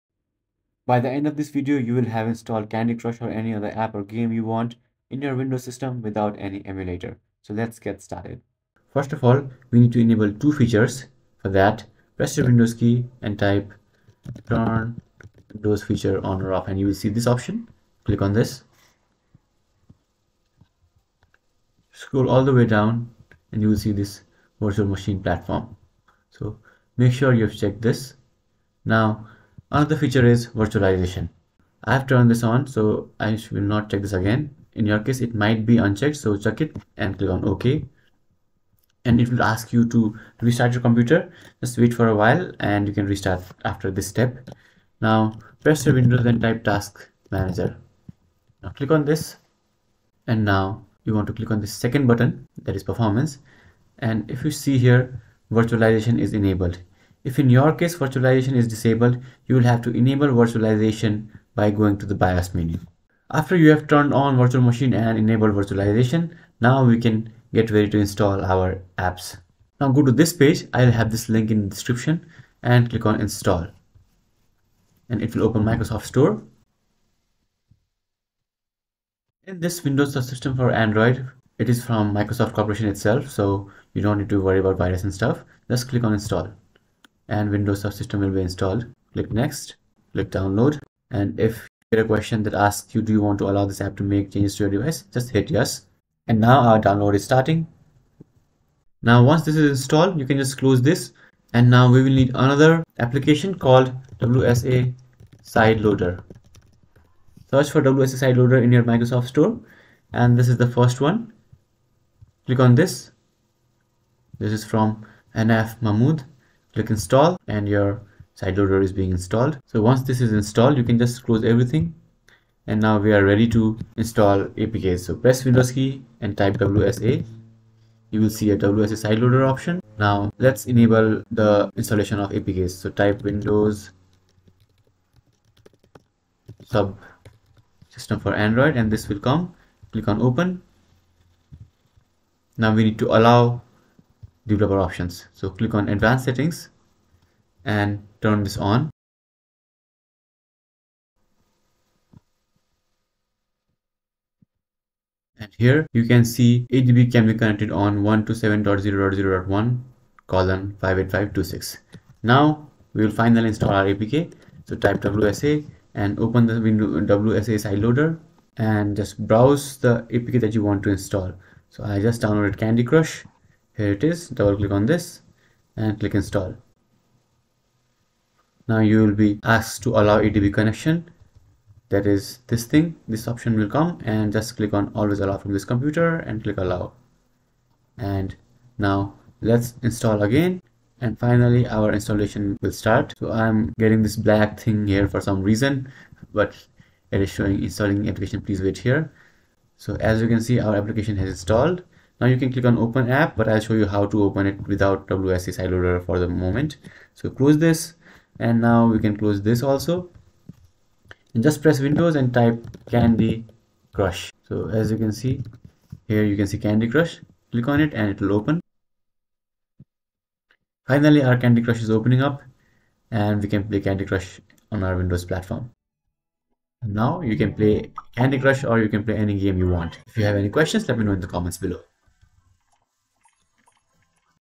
Install Android Apps/Games on Windows 11/10 Without Emulator 🔥 (2023 Update)
In this video, I'll show you how to Install Apps or Install Games on Windows 11 or Windows 10 without installing any emulator. We use Windows subsystem for Android to Install Apps and Games on Windows PC.

After watching this video, you'll be able to install games or apps on your windows 10 or windows 11 PC without installing emulator.

Video Topics:
- Enable Virtualization and Virtual Machine
- Install Windows Subsystem for Android
- Install WSA Sideloader
- Enable developer option on WSA
- Install Candy Crush on Windows 11
- Install Games on Windows without Emulator

By the end of the video, we will have enabled WSA, installed wsa sideloader, and installed android games on windows 11.